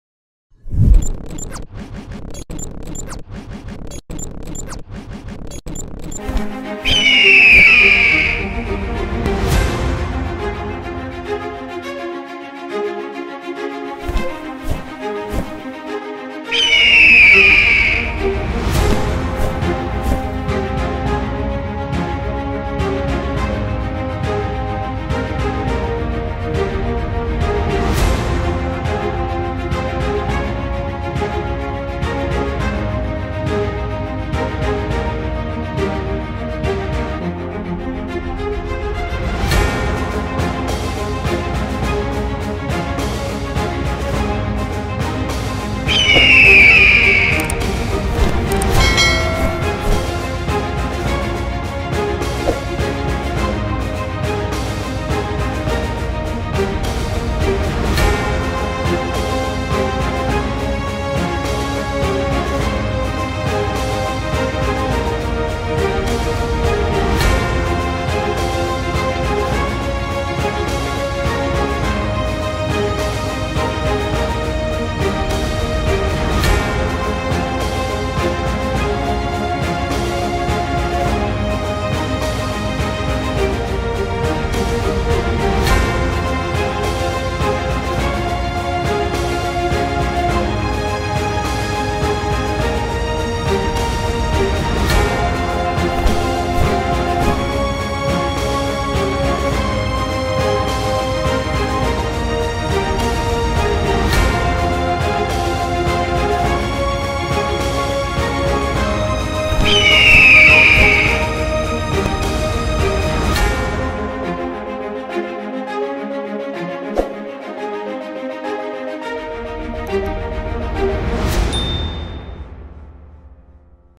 Video Material Eagle in Green Screen 4k#eagle
This Eagle Green Screen can be easily used in your own videos just a simple drag and drop. Whether you're a professional video editor or just starting out, these Material will help you bring your projects to the next level and you are able to download this for free.